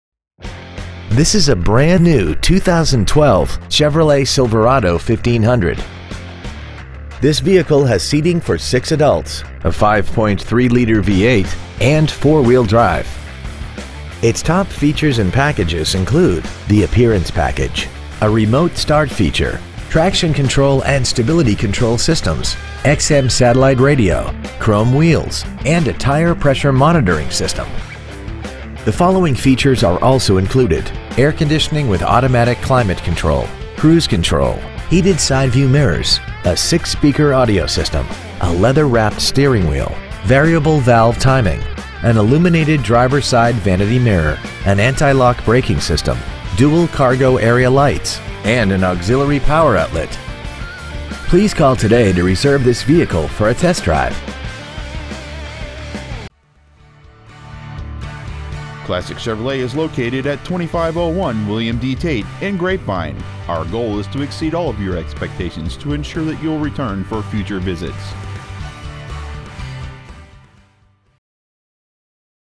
2012 Chevrolet Silverado 1500 Ft. Worth TX
Year : 2012
 Make : Chevrolet
 Model : Silverado 1500
 Engine : Gas/Ethanol V8 5.3L/323
 Trans . : Automatic
 Exterior : Black
 Miles : 3

 2012 Chevrolet Silverado 1500 Grapevine TX